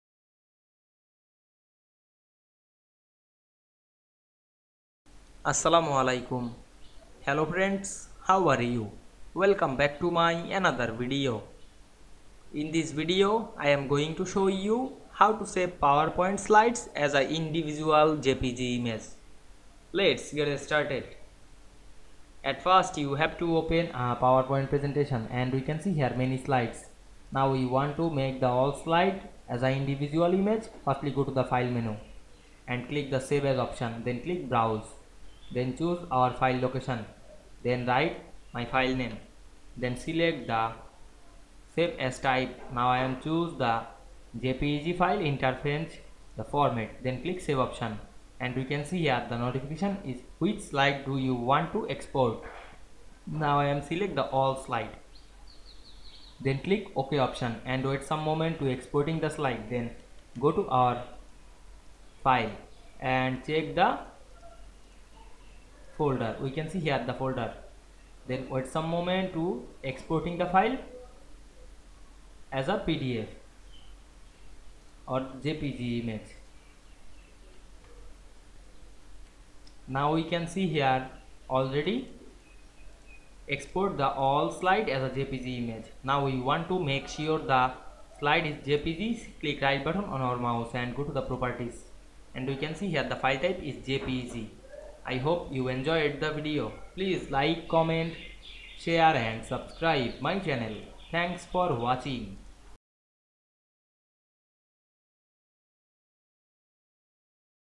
How to save PowerPoint slides as individual JPEG images
Assalamu Walaikum,
In this video I will show you, How to save PowerPoint slides as individual JPEG images.  Let's get started.